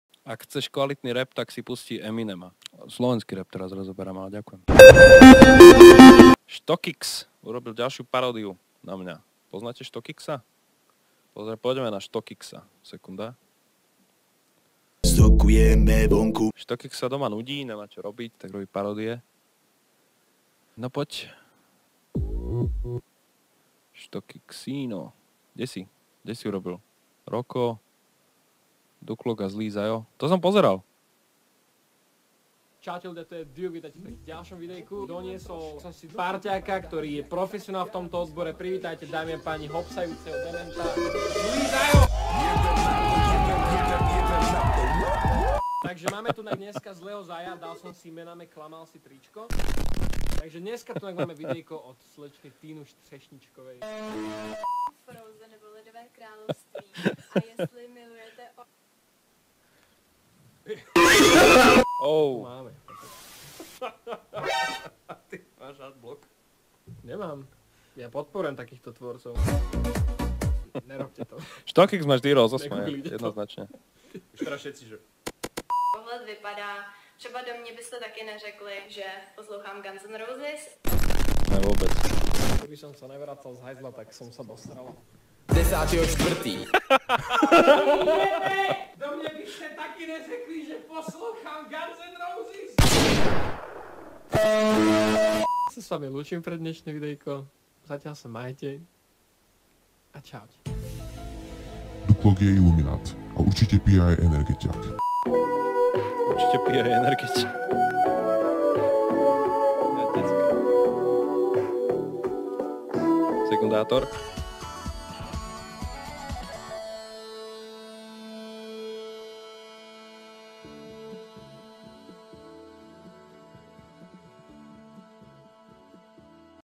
ZLÝ ZAJO REAGUJE NA MOJE VIDEO :D
Na streame na YouTube si Zlý Zajo pustil moje video. Tu máte záznam zo streamu, kde na mňa reagoval. Ešte raz Vám všetkým ďakujem. :D